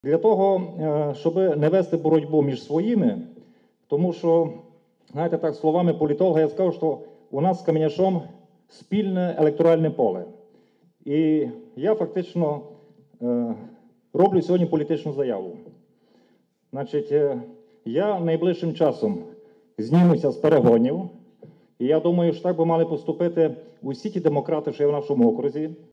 Микола Вегеш:"Так повинні діяти усі демократи"
Микола Вегеш про своє рішення зняти кандидатуру на перегонах 2012. Всі члени нашої команди, кандидати на виборах по шести округах Закарпаття, завжди ставили спільні інтереси вище особистих.
Сьогодні Микола Вегеш, який балотується по 70 виборчому округу, прийняв рішення зняти свою кандидатуру на користь іншого представника нашої закарпатської команди Олександра Кеменяша.
Підтримую таке рішення і вважаю, що Кеменяш буде достойно представляти Закарпаття у Верховній Раді. Водночас, я впевнений, що Микола Вегеш зможе реалізувати себе як депутат обласної ради, науковець, людина авторитетна для всіх закарпатців.